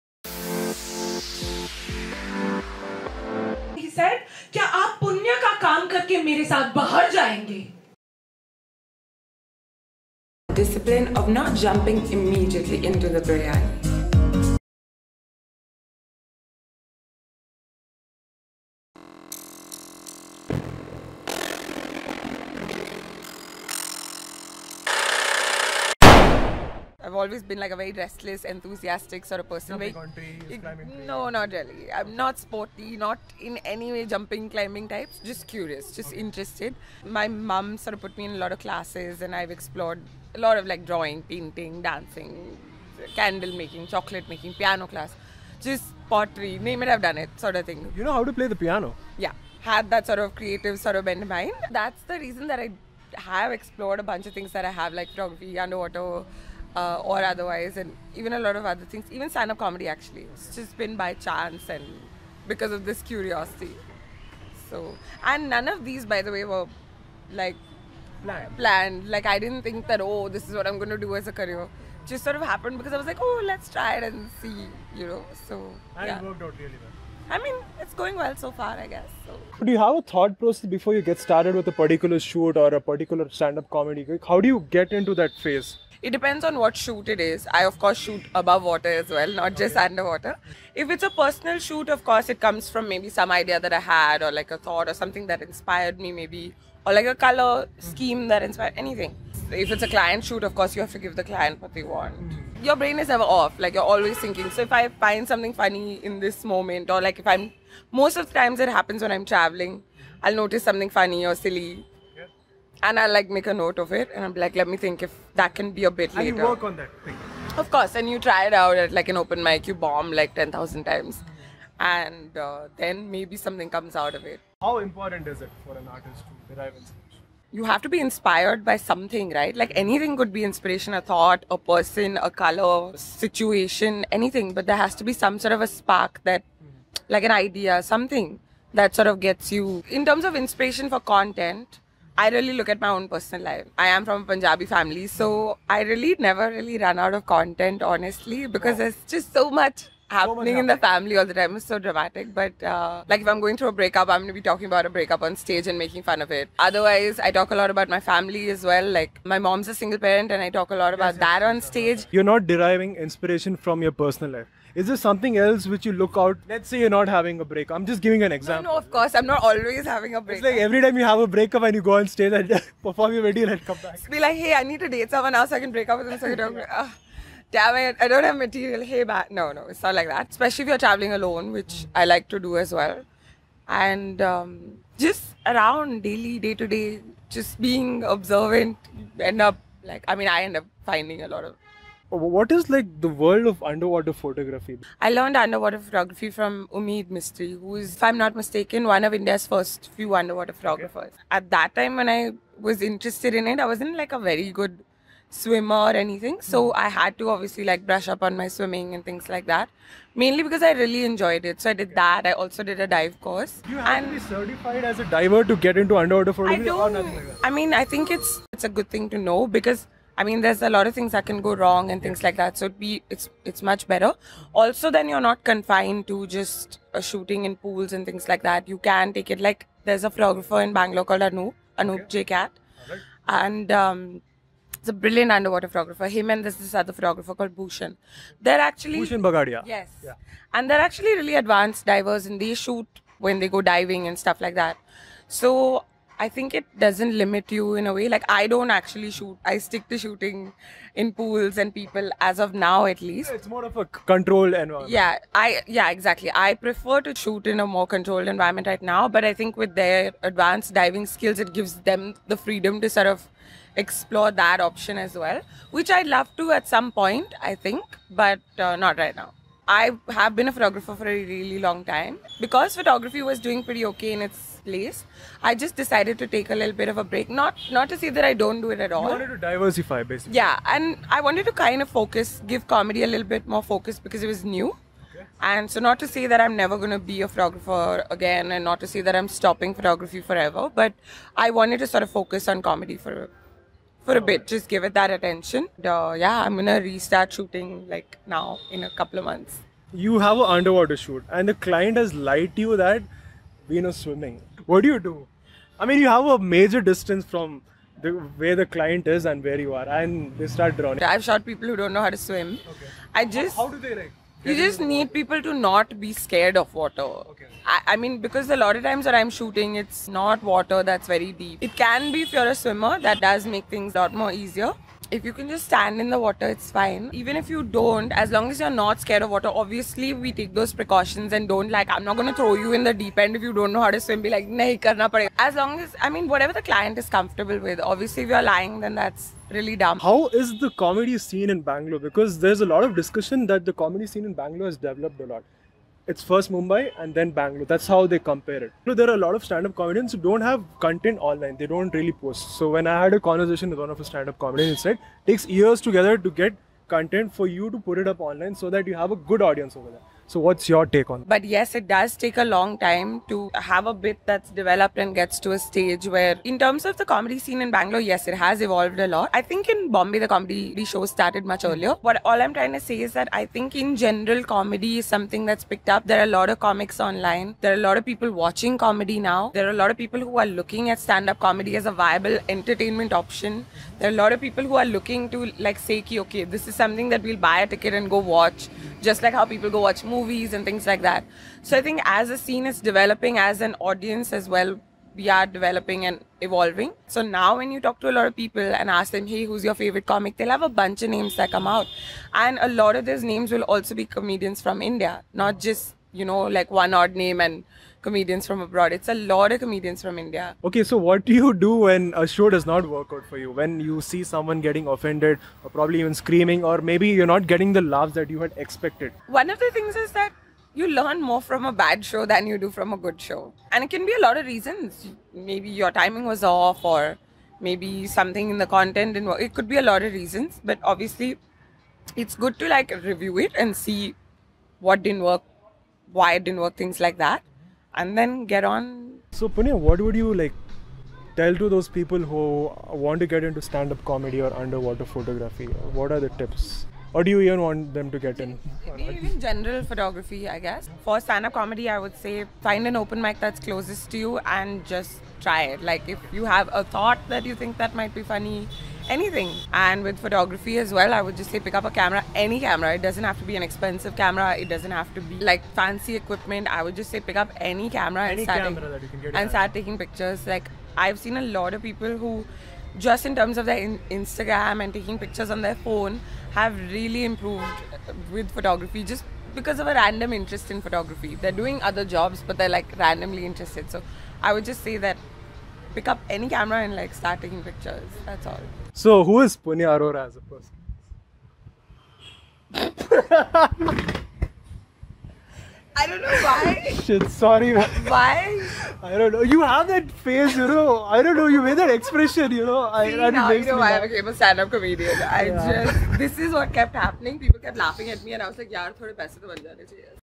Who is Punya Arora?|Stand Up Comedian|Underwater photographer|Youcollabor show Ep.8
Punya Arora is a Benguluru based stand up comedian and underwater photographer, in this interview we get to know her better as a person plus a sneek peep into her professional life as well.

Filmed by :- Ruban Kumar